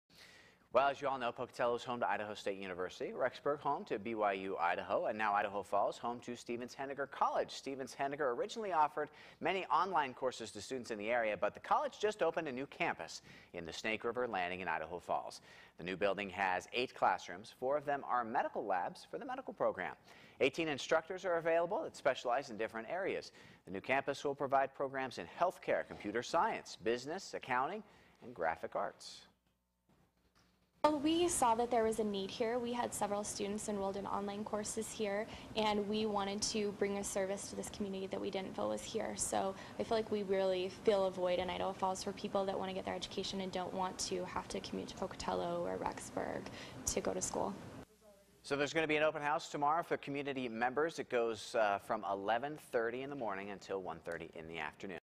Stevens-Henager Opens New Campus in Idaho Falls
As you all know, Pocatello is home to Idaho State University, Rexburg is home to Brigham Young University -- Idaho and now Idaho Falls is home to Stevens -- Henager College.